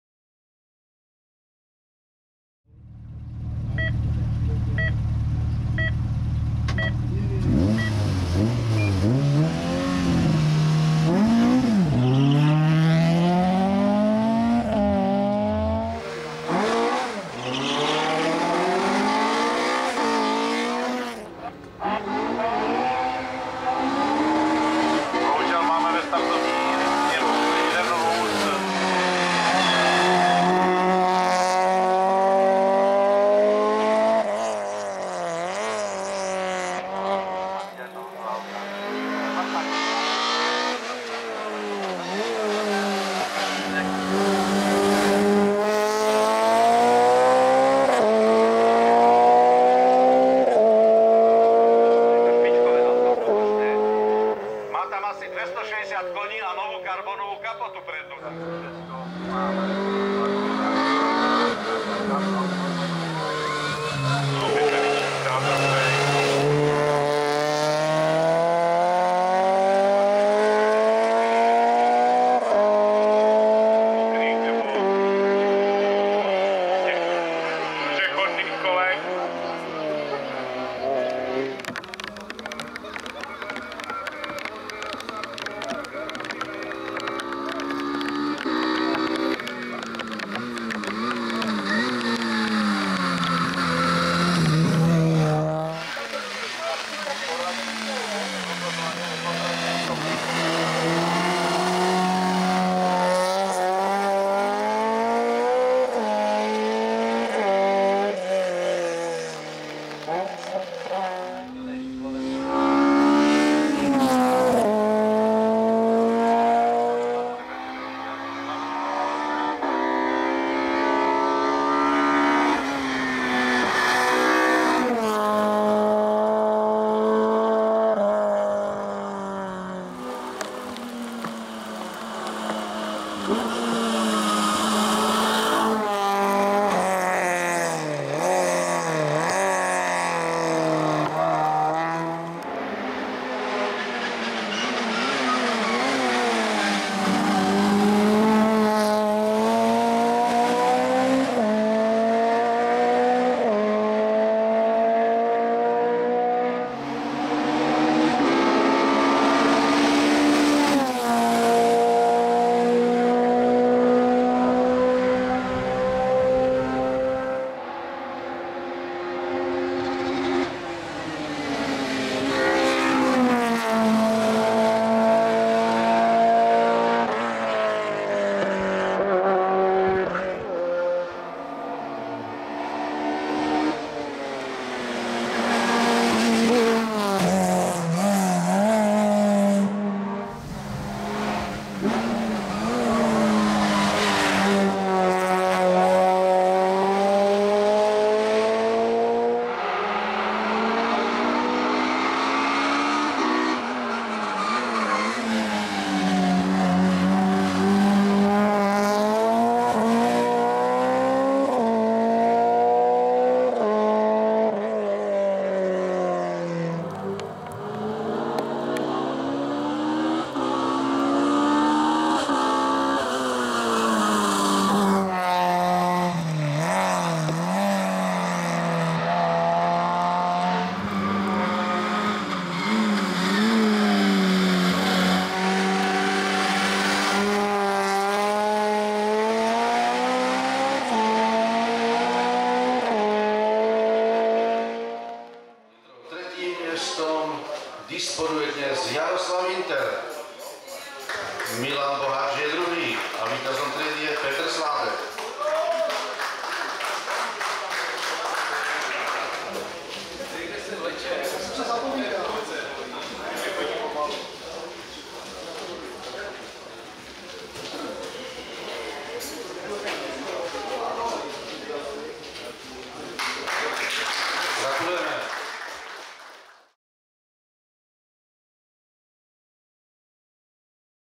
Petr Sládek - Honda Civic 1,8 i-VT - III. GMS Albrechtický kopec 2022 - Albrechtice u Lanškrouna
Petr Sládek si s vozem Honda Civic 1,8 i-VT dojel pro vítězství ve třídě E1H-2000 v závodech do vrchu III. GMS Albrechtický kopec, II. Memoriál Jiřího Lose – Albrechtice u Lanškrouna 2022 v seriálu Autoklub Česká trofej, a to i přes nezajištěnou přední kapotu na svém závodním voze.